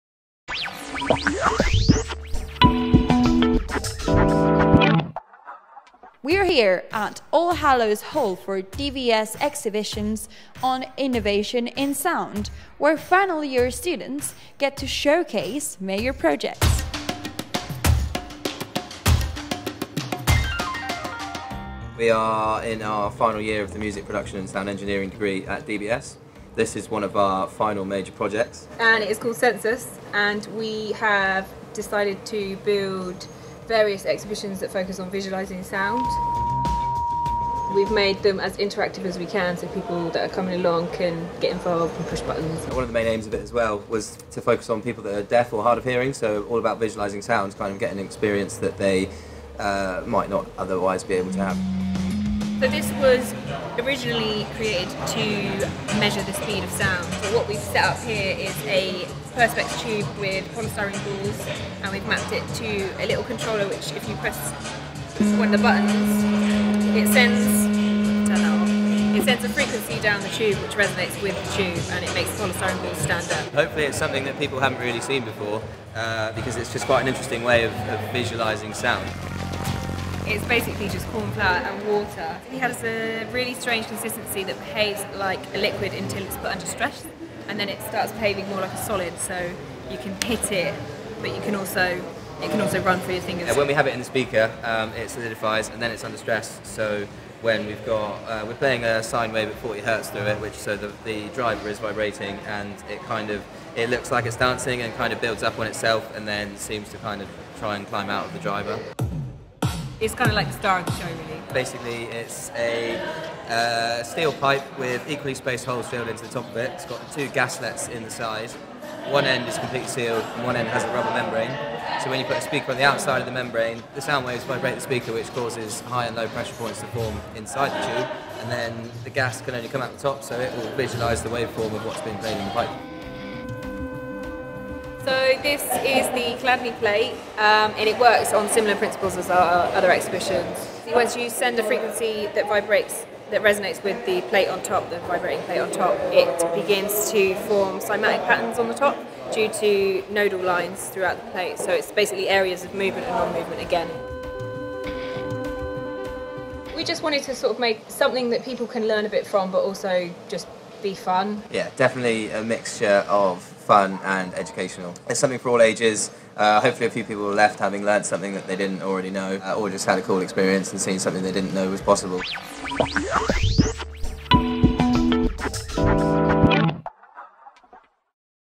Student Stories - SENSUS Audio Installations
We catch up with two of our Bristol degree students and take a look at their amazing SENSUS audio installation project, aiming to visualise sound in interesting ways including a Rubins Tube, Chladni style plates and more...

Learn more about degree and diploma courses for ages 16+  at dbsmusic.co.uk